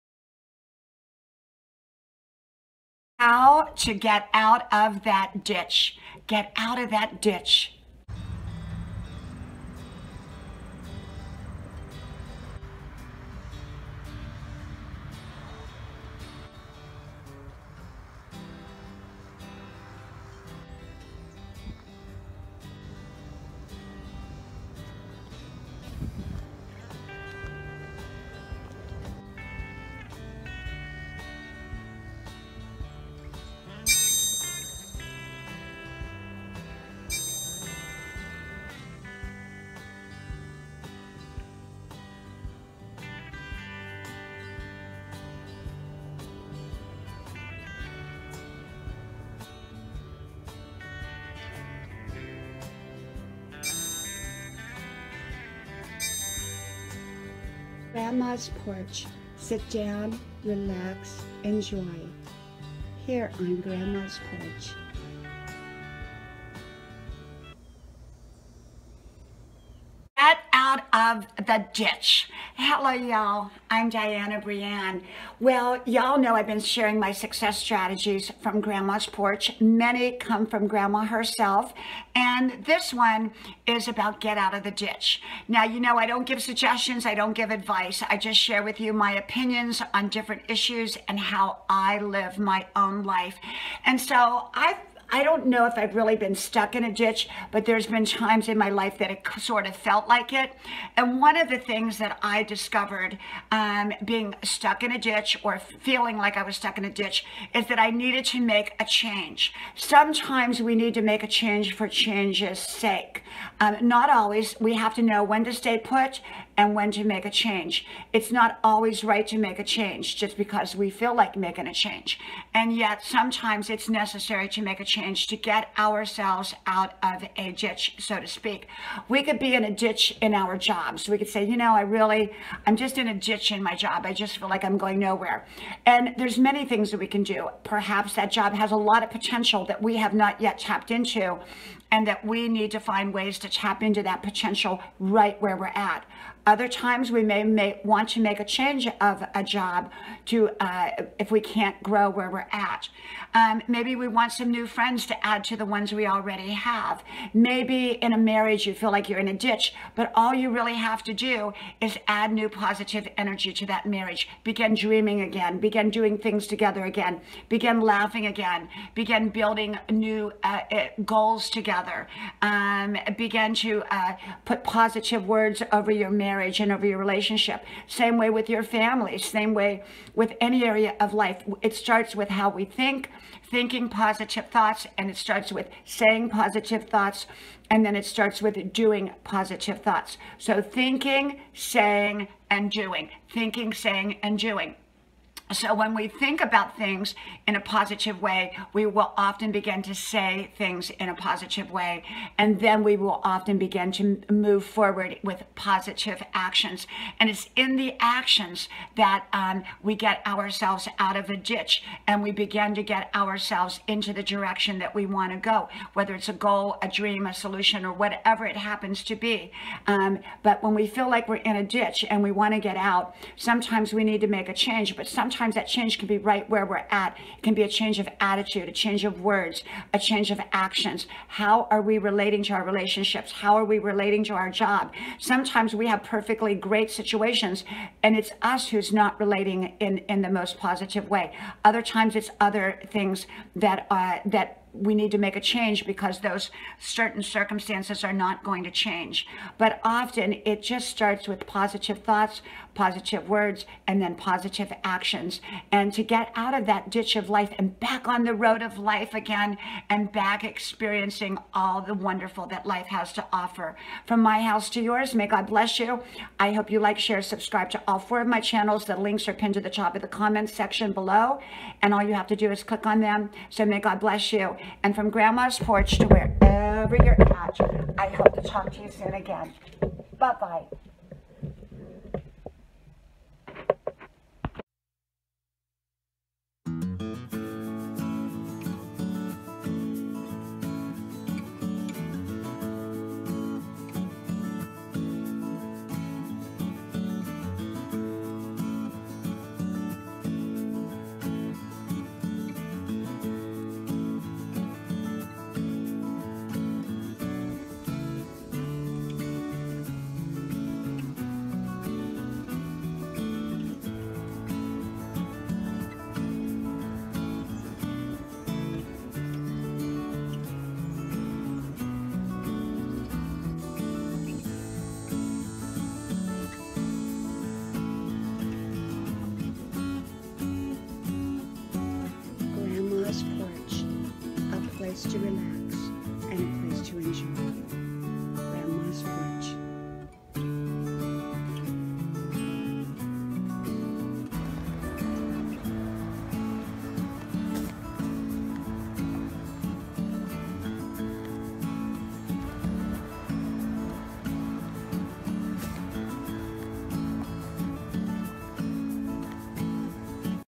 WHEN YOU ARE STUCK IN A DITCH! HOW TO GET OUT OF THE DITCH! MAKING POSITIVE SUCCESSFUL DECISIONS!
HOW TO GET UNSTUCK AND BACK ON THE ROAD OF LIFE AGAIN.